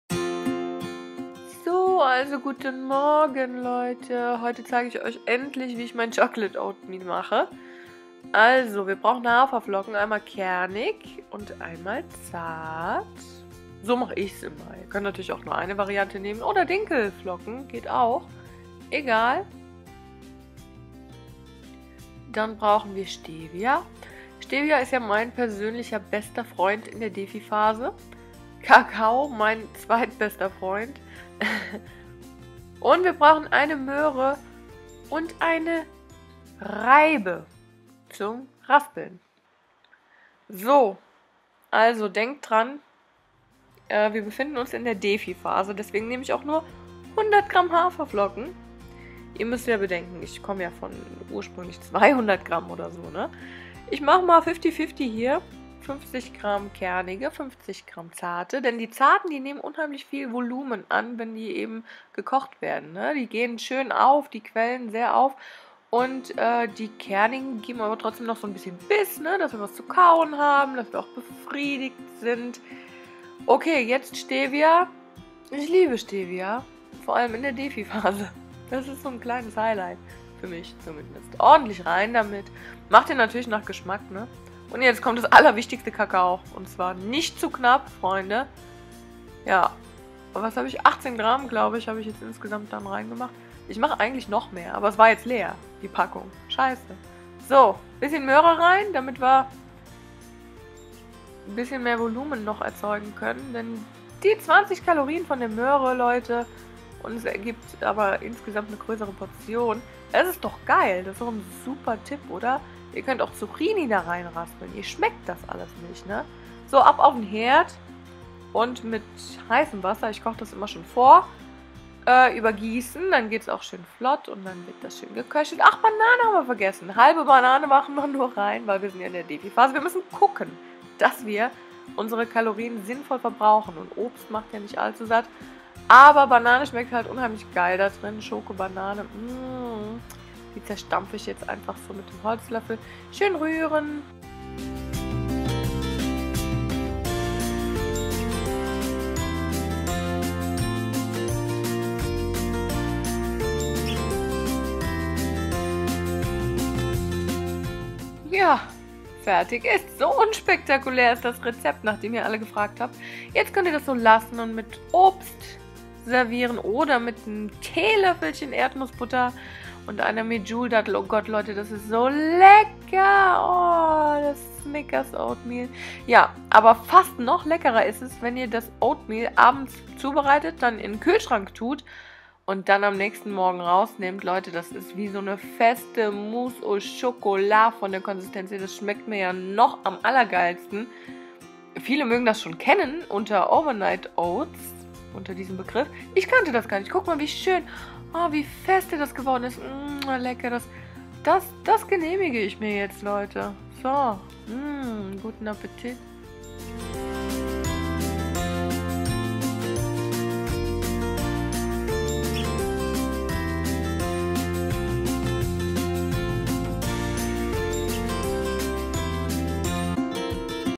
Skinny Chocolate Oatmeal - Mein LECKERES Frühstück in der Defiphase
Die klassische, NOCH leckerere Variante für die NICHT-Defiphase:

200 g Haferflocken ;)
300 ml Hafer-Vanillemilch
Reissirup ohne Ende
2 Bananen
Kakao ohne Ende
Medjouldatteln ohne Ende
Erdnussbutter ohne Ende

SPARE 5 € mit meinem CODE: SBRI01